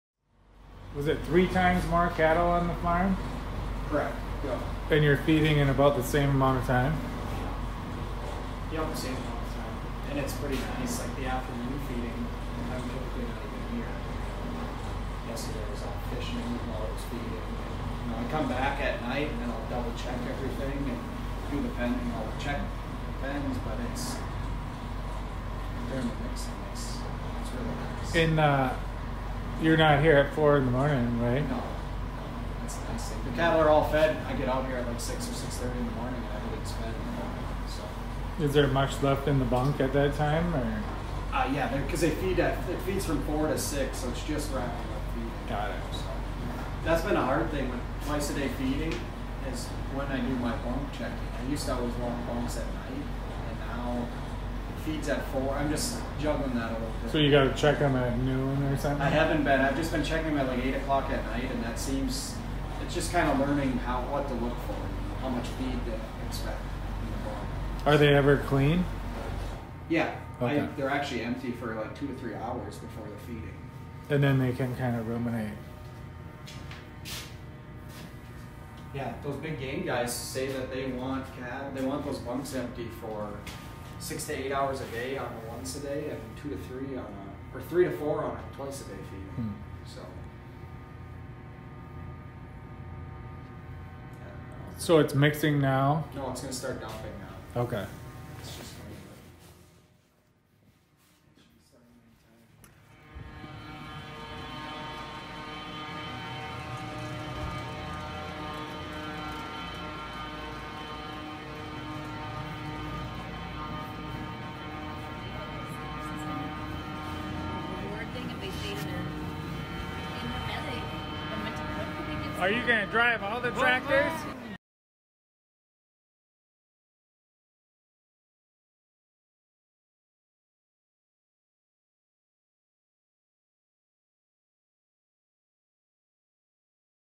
Robotic Automatic Cattle Feeding System in Beef Barn by Hanson silo and Valmetal
This video shows a complete cattle feeding system with no human intervention. Famer feeds 600 head in the cow slatted floor barn with 12' deep pit.  200 more cattle in bed pack barn outside in TMR trailer mixer.  Hansonsilo a Better way to feed the world.